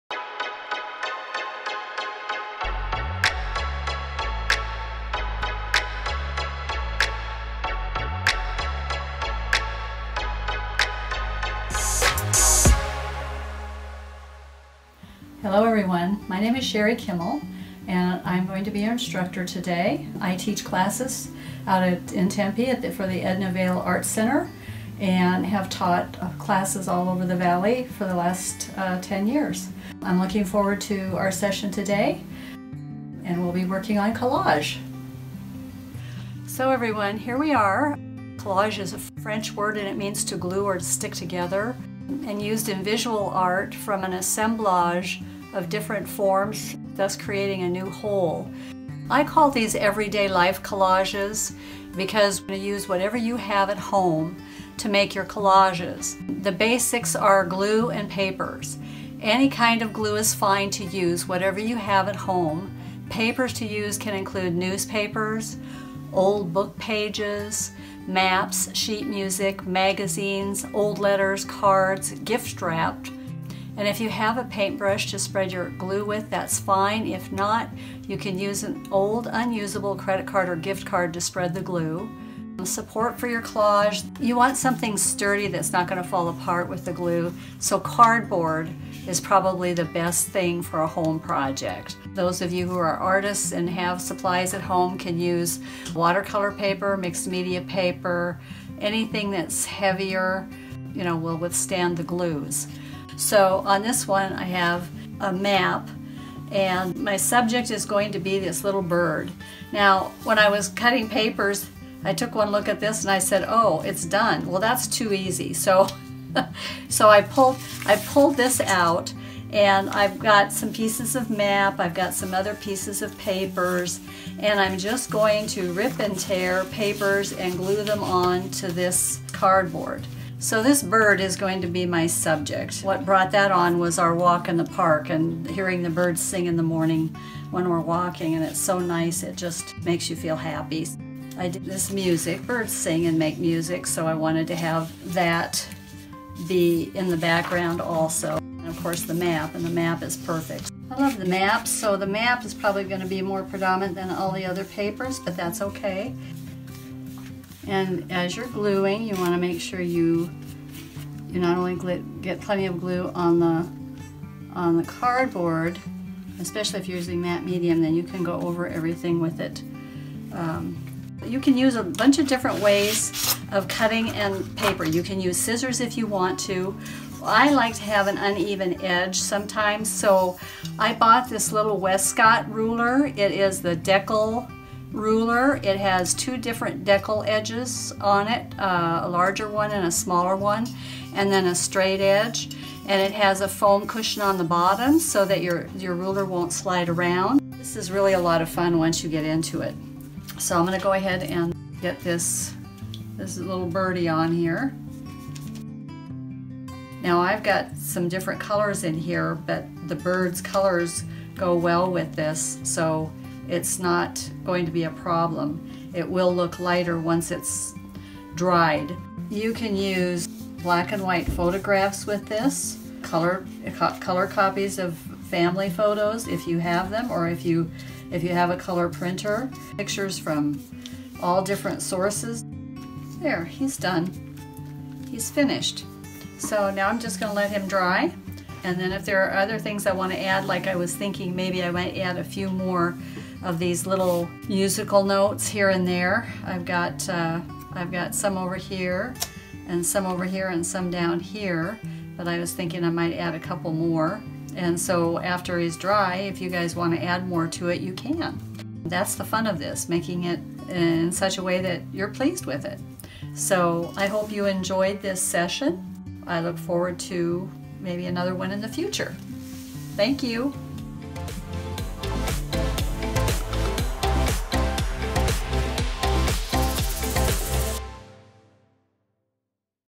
ARTS - Everyday Collage
Sherrie Kimmel shows you some great ways to get creative at home.